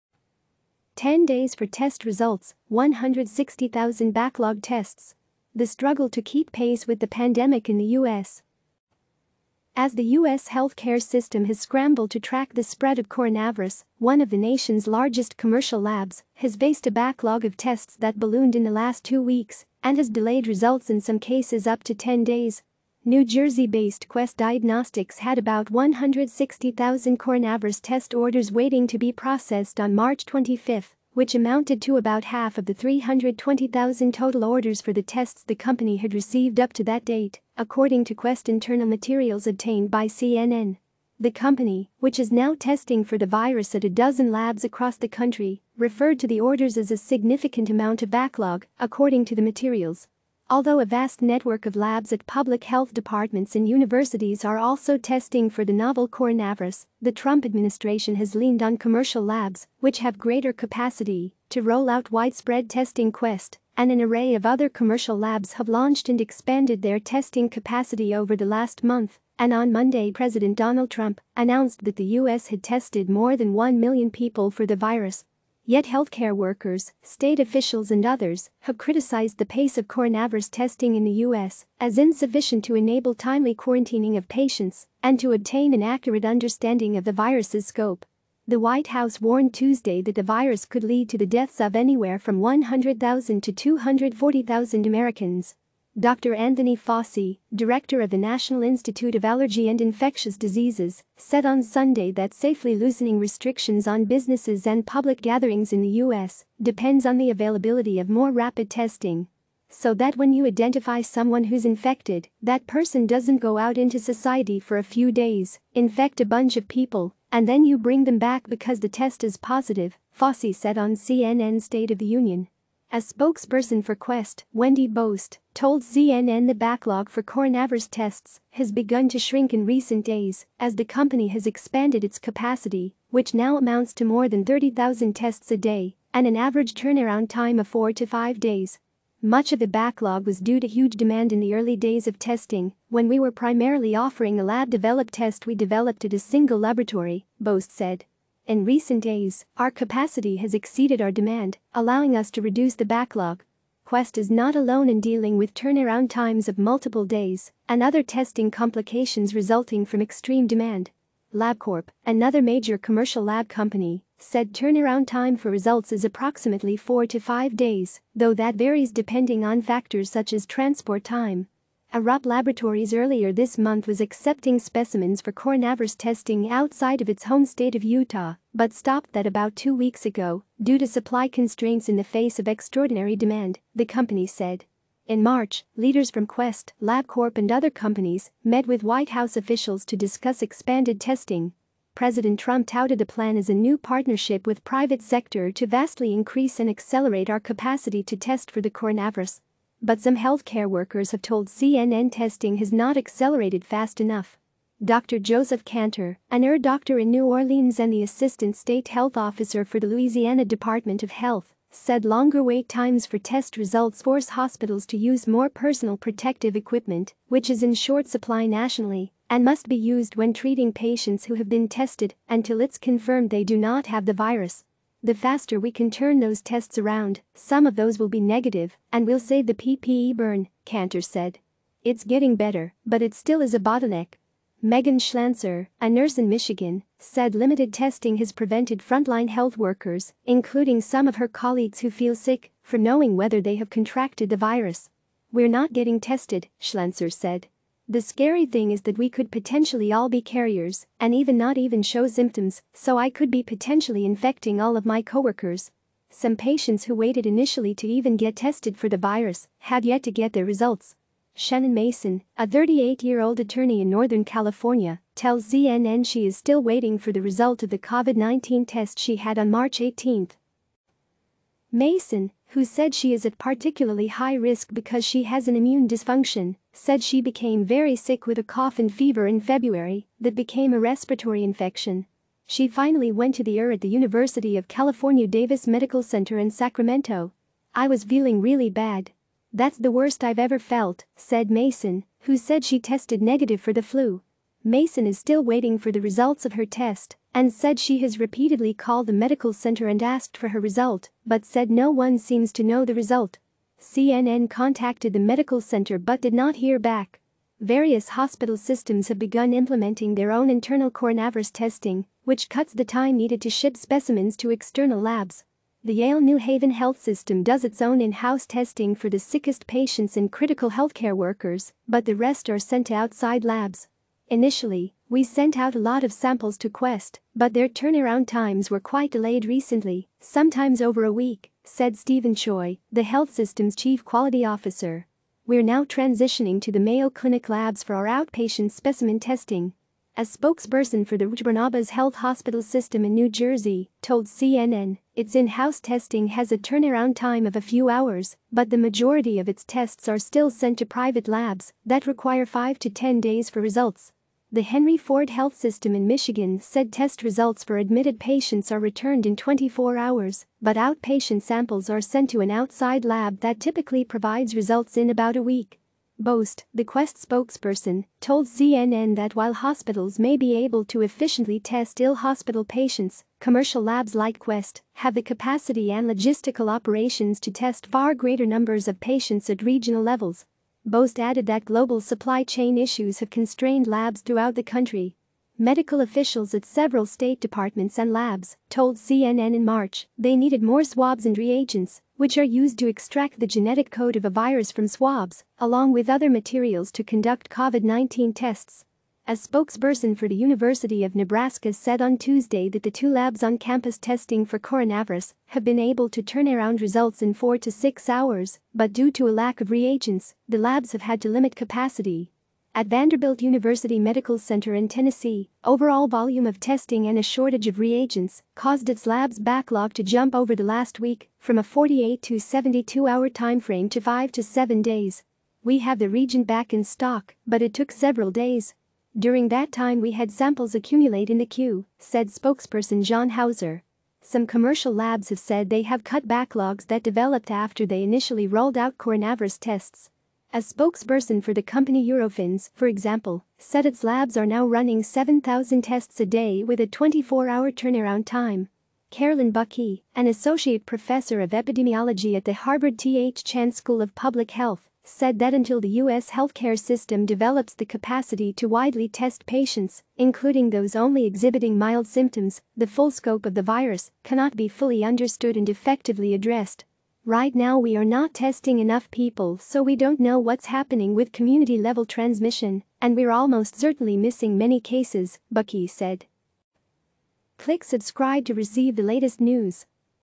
10 days for test results, 160,000 backlogged tests: The struggle to keep pace with the pandemic ...
As the US health care system has scrambled to track the spread of coronavirus, one of the nation's largest commercial labs has faced a backlog of tests that ballooned in the last two weeks, and has delayed results in some cases up to 10 days.

New Jersey-based Quest Diagnostics had about 160,000 coronavirus test orders waiting to be processed on March 25, which amounted to about half of the 320,000 total orders for the tests the company had received up to that date, according to Quest internal materials obtained by CNN.

The company, which is now testing for the virus at a dozen labs across the country, referred to the orders as a "significant amount of backlog," according to the materials.

Although a vast network of labs at public health departments and universities are also testing for the novel coronavirus, the Trump administration has leaned on commercial labs, which have greater capacity, to roll out widespread testing

Quest and an array of other commercial labs have launched and expanded their testing capacity over the last month, and on Monday President Donald Trump announced that the US had tested more than 1 million people for the virus.

Yet healthcare workers, state officials and others have criticized the pace of coronavirus testing in the US as insufficient to enable timely quarantining of patients and to obtain an accurate understanding of the virus' scope. The White House warned Tuesday that the virus could lead to the deaths of anywhere from 100,000 to 240,000 Americans.

Dr. Anthony Fauci, director of the National Institute of Allergy and Infectious Diseases, said on Sunday that safely loosening restrictions on businesses and public gatherings in the US depends on the availability of more rapid testing.

"So that when you identify someone who's infected, that person doesn't go out into society for a few days, infect a bunch of people, and then you bring them back because the test is positive," Fauci said on CNN's "State of the Union."

A spokesperson for Quest, Wendy Bost, told CNN the backlog for coronavirus tests has begun to shrink in recent days as the company has expanded its capacity, which now amounts to more than 30,000 tests a day and an average turnaround time of four to five days.

"Much of the backlog was due to huge demand in the early days of testing when we were primarily offering a lab-developed test we developed at a single laboratory," Bost said. "In recent days, our capacity has exceeded our demand, allowing us to reduce the backlog."

Quest is not alone in dealing with turnaround times of multiple days and other testing complications resulting from extreme demand.

LabCorp, another major commercial lab company, said turnaround time for results is approximately four to five days, though that varies depending on factors such as transport time.

ARUP Laboratories earlier this month was accepting specimens for coronavirus testing outside of its home state of Utah but stopped that about two weeks ago due to "supply constraints in the face of extraordinary demand," the company said.

In March, leaders from Quest, LabCorp and other companies met with White House officials to discuss expanded testing. President Trump touted the plan as "a new partnership with private sector to vastly increase and accelerate our capacity to test for the coronavirus."

But some health care workers have told CNN testing has not accelerated fast enough.

Dr. Joseph Kanter, an ER doctor in New Orleans and the Assistant State Health Officer for the Louisiana Department of Health, said longer wait times for test results force hospitals to use more personal protective equipment, which is in short supply nationally and must be used when treating patients who have been tested until it's confirmed they do not have the virus.

"The faster we can turn those tests around, some of those will be negative and we'll save the PPE burn," Kanter said. "It's getting better, but it still is a bottleneck."

Megan Schlanser, a nurse in Michigan, said limited testing has prevented frontline health workers, including some of her colleagues who feel sick, from knowing whether they have contracted the virus.

"We're not getting tested," Schlanser said. "The scary thing is that we could potentially all be carriers and even not even show symptoms... so I could be potentially infecting all of my coworkers."

Some patients who waited initially to even get tested for the virus have yet to get their results.

Shannon Mason, a 38-year-old attorney in northern California, tells CNN she is still waiting for the result of the Covid-19 test she had on March 18.

Mason, who said she is at particularly high risk because she has an immune dysfunction, said she became very sick with a cough and fever in February that became a respiratory infection.

She finally went to the ER at the University of California-Davis Medical Center in Sa